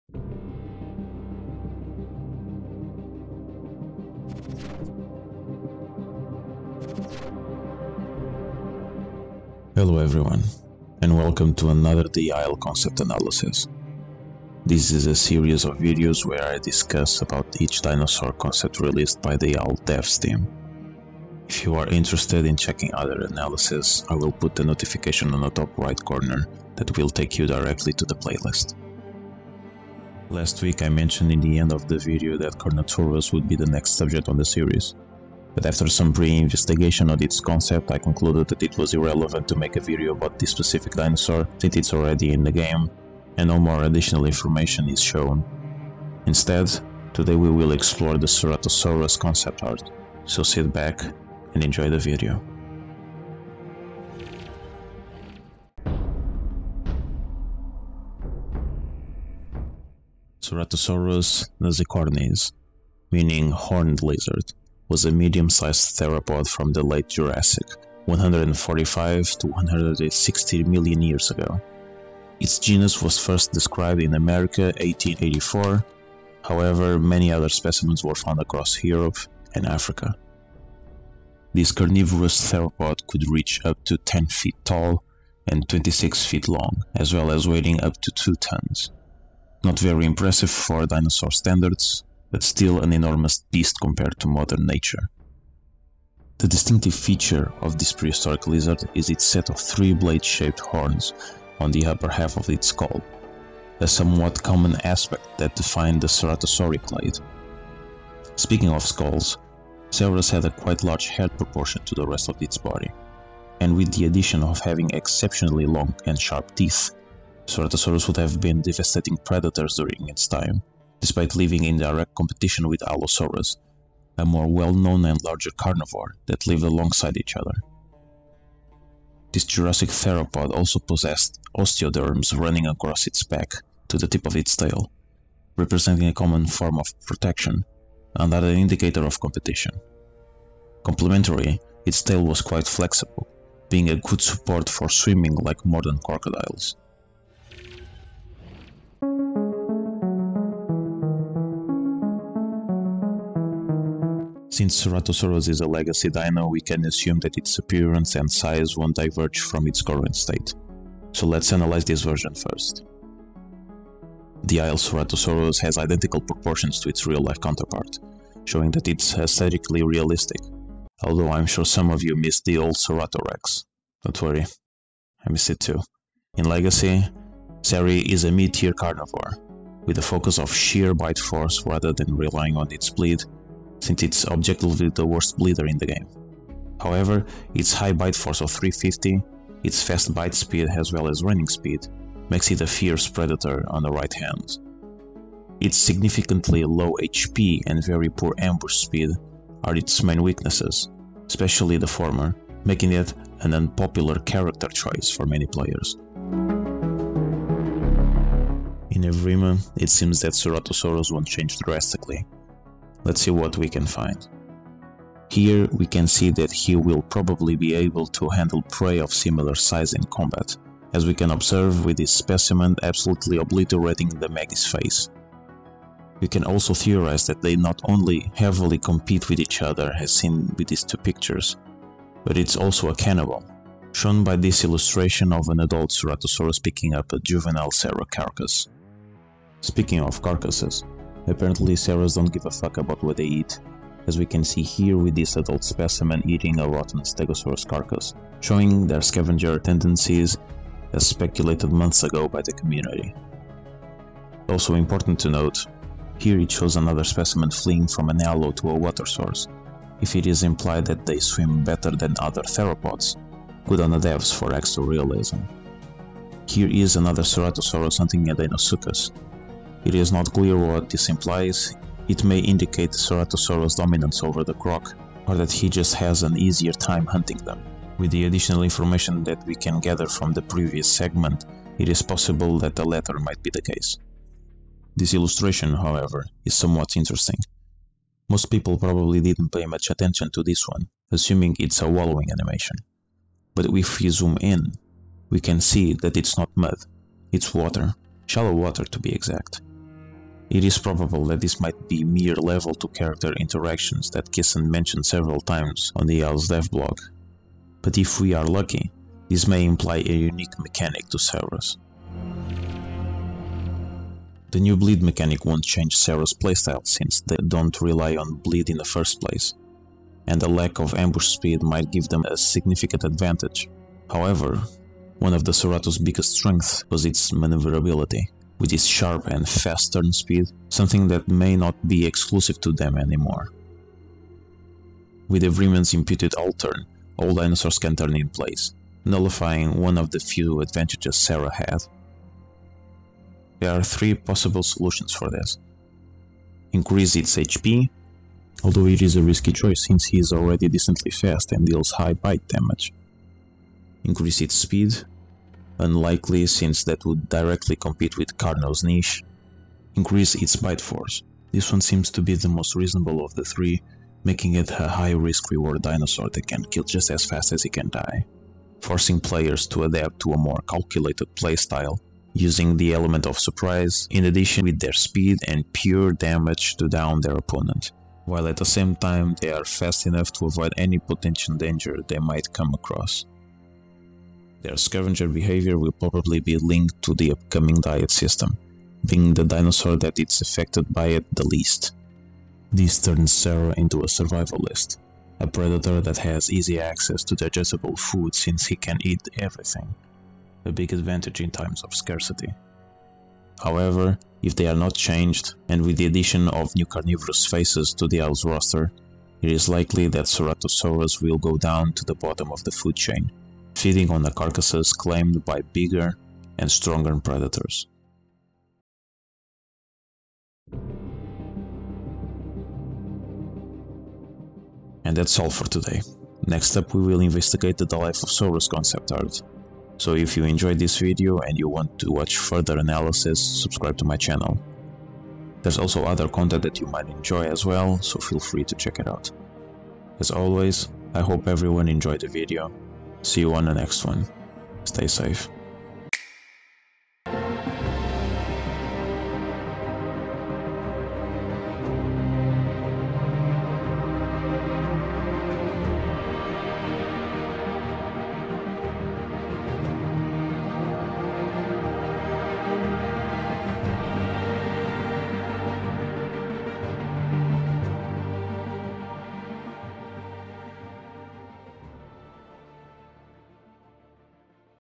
The Isle Concept Analysis | Ceratosaurus
Hello everyone, and welcome to the second The Isle Concept Analysis! This is a series I will be making for the following weeks where I explore and analyse all the dinosaur concepts that the devs have been releasing since 2020. Today we are going to discuss the Ceratosaurus.

Chapters:
0:00 Intro
0:53 Original
2:26 Asset
5:12 Speculation
7:00 Outro